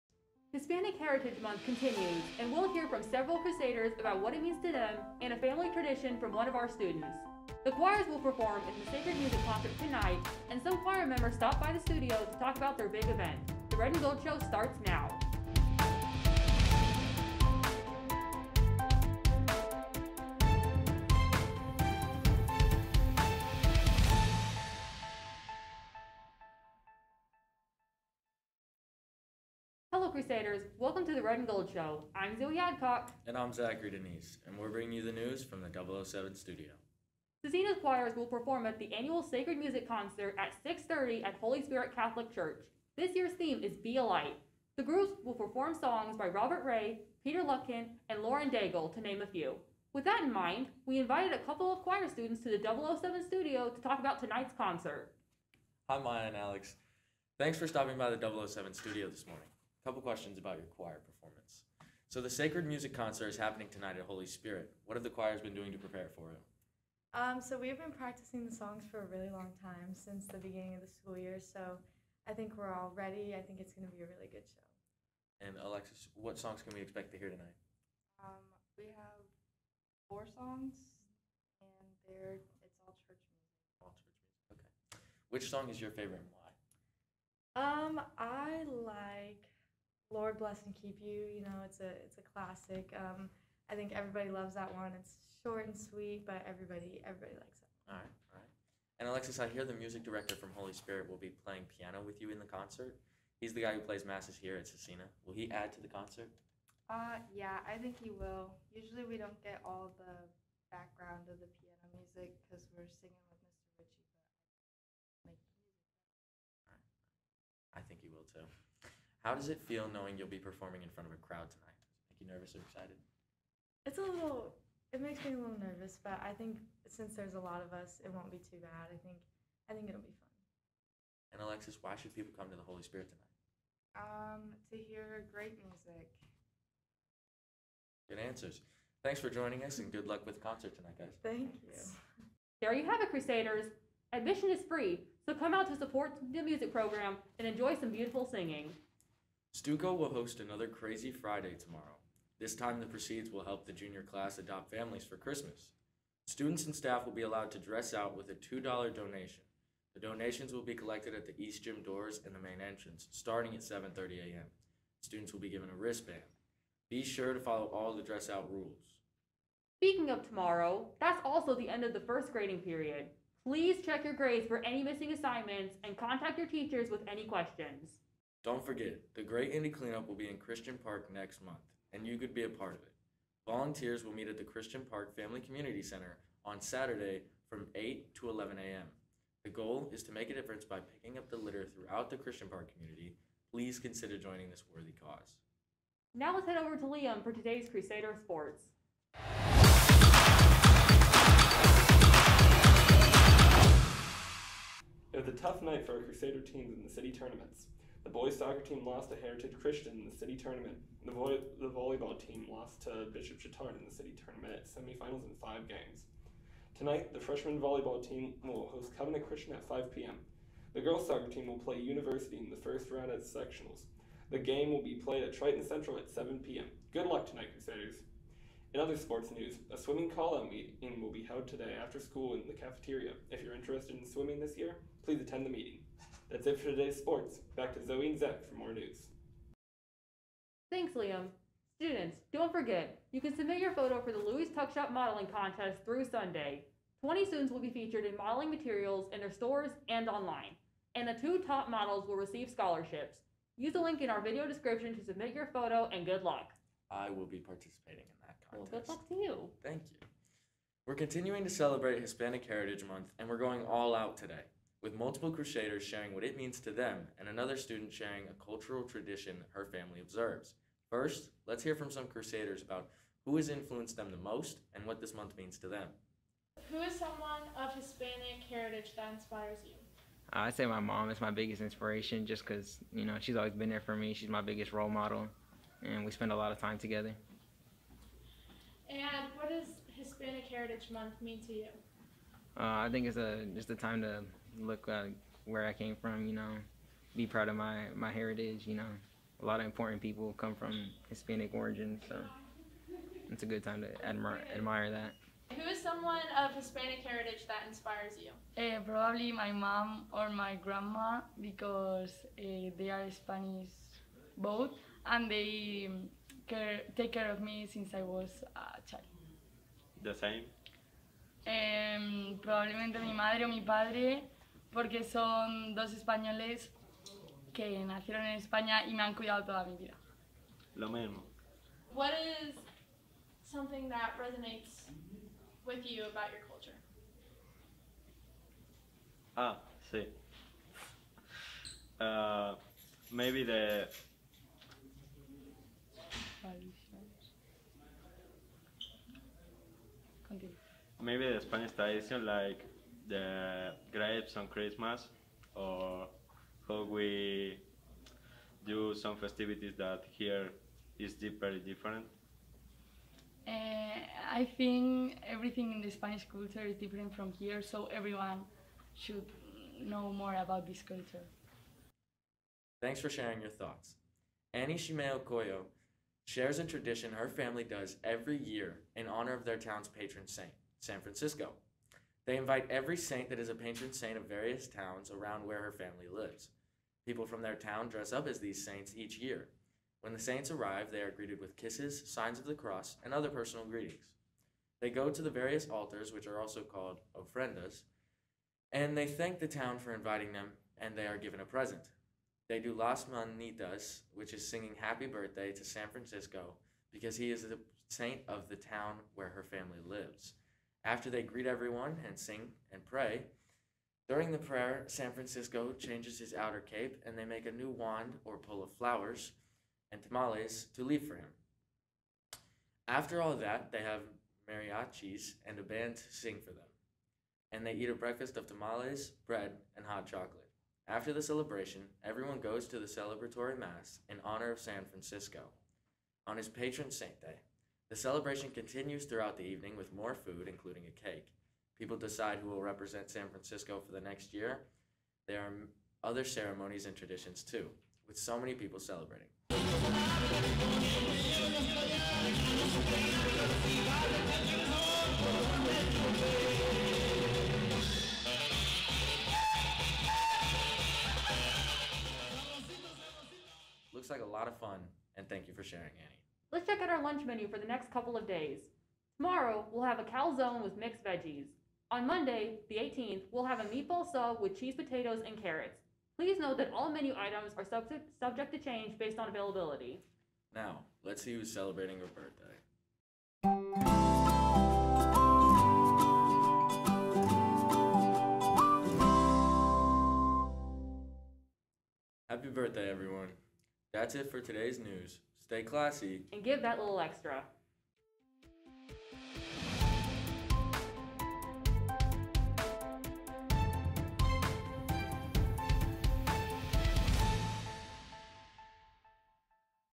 RG 10-7-21
Scecina's daily student news show: Submit photo for Louie's Tux Shop modeling opportunity